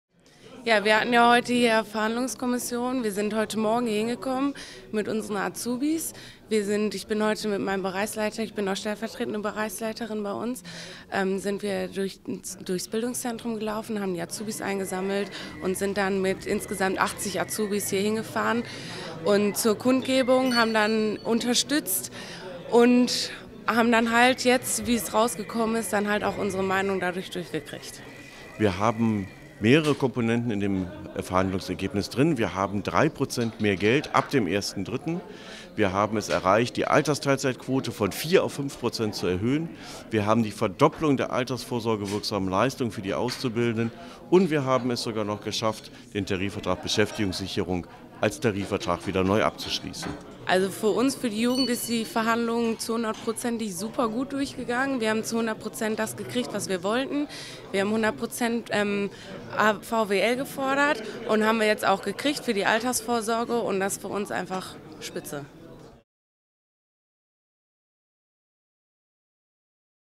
Tarifrunde Stahl 2013: Verhandlungsergebnis. 3 Prozent mehr Geld für Stahlarbeiter
Nach 10-stündiger Verhandlung einigten sich die IG Metall und die Stahlarbeitgeber am frühen Mittwochmorgen in Düsseldorf auf ein Verhandlungsergebnis für die 75.000 Beschäftigten der Eisen- und Stahlindustrie in Nordrhein-Westfalen, Niedersachsen und Bremen. Ab dem 1. März 2013 erhalten die Stahlarbeiter um 3 Prozent höhere Löhne, Gehälter und Ausbildungsvergütungen. Der Vertrag hat eine Laufzeit von 15 Monaten bis zum 31. Mai 2014. Die Quote der Anspruchsberechtigten auf Altersteilzeit wird für diese Laufzeit von 4 auf 5 Prozent angehoben. Beschäftigte, die künftig einen Altersteilzeitvertrag abschließen, bekommen einen 2-prozentigen statt bisher 1-prozentigen Inflationsausgleich für die Abfindungszahlungen zum Rentenübergang. Für die Auszubildenden, die ab sofort Altersvorsorgewirksame Leistungen in Anspruch nehmen, konnte eine Verdoppelung des gegenwärtigen Arbeitgeberbeitrags auf 26,59 € erreicht werden. Der Anfang des Jahres ausgelaufene Tarifvertrag zur Beschäftigungssicherung wurde verlängert.